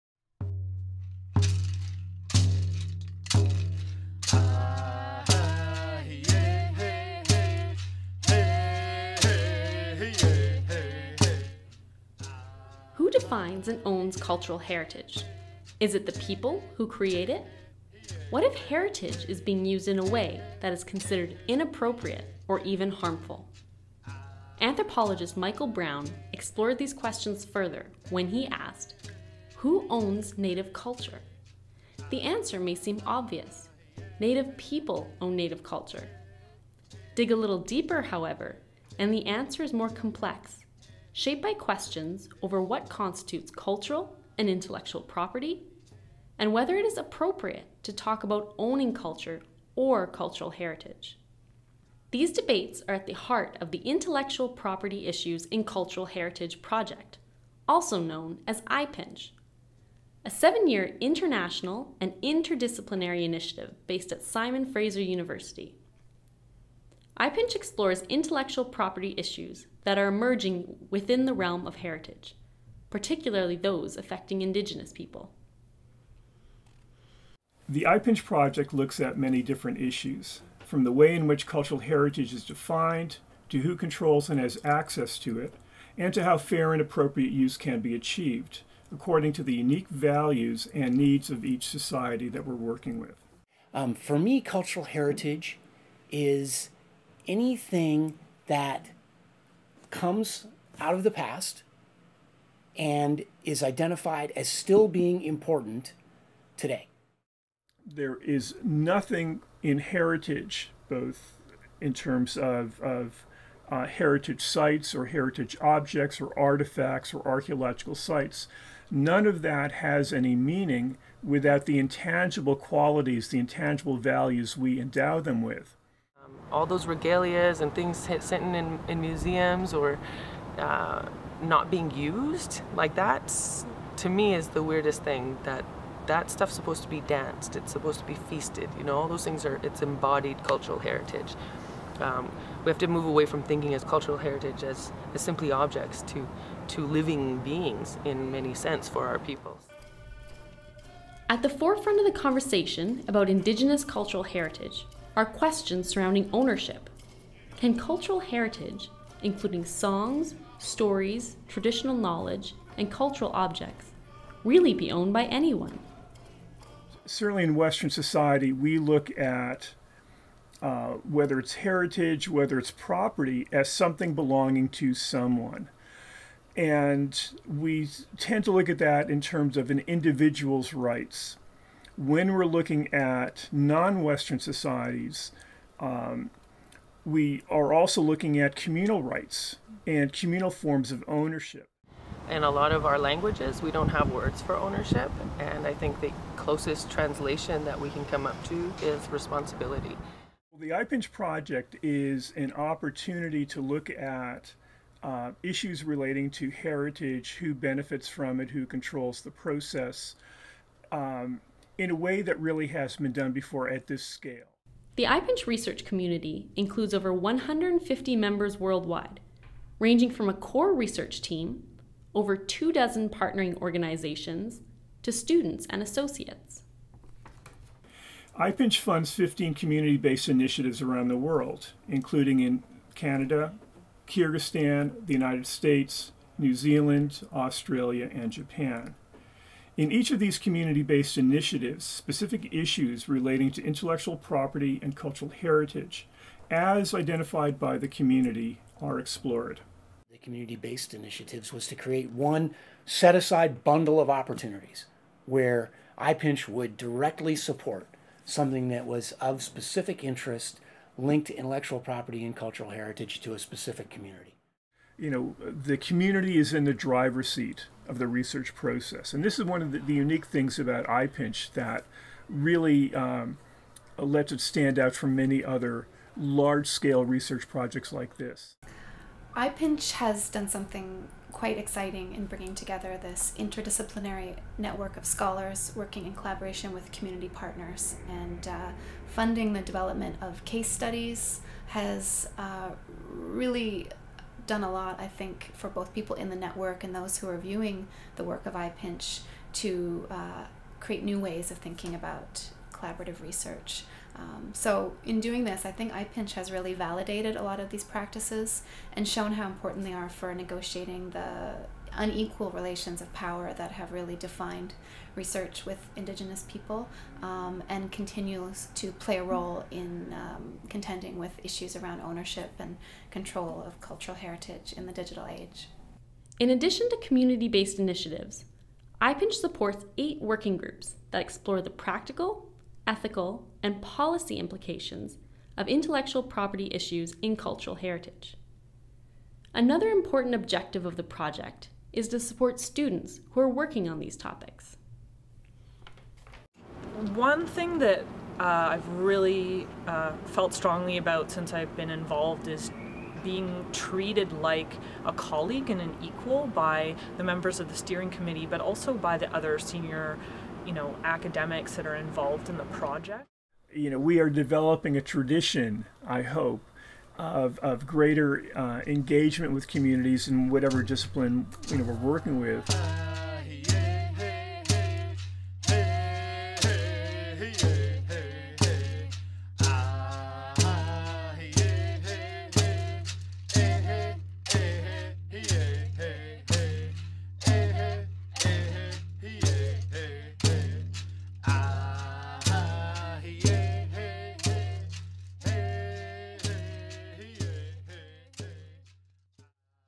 Introduction to IPinCH
Who defines and owns cultural heritage? Is it the people who create it? What if heritage is being used in ways that are considered inappropriate, or even harmful?

These questions are at the heart of the IPinCH project.

Video created by Aynur Kadir (IPinCH RA, PhD student in the School of Interactive Arts and Technology, SFU Surrey) and Alexa Walker (IPinCH RA, MA student in the Department of Archaeology, SFU).